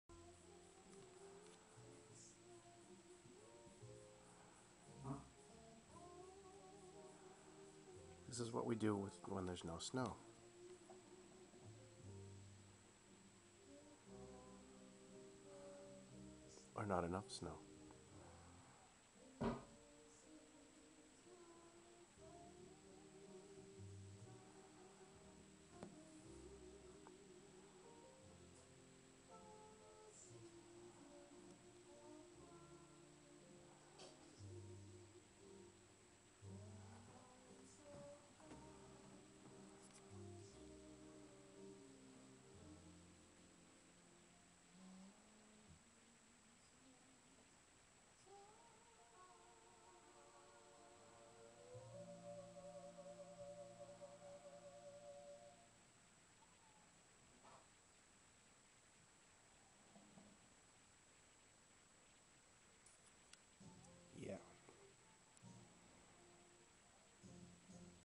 Sledding with no snow.mpg
When there's not enough snow to sled, the boys find a way to make it work!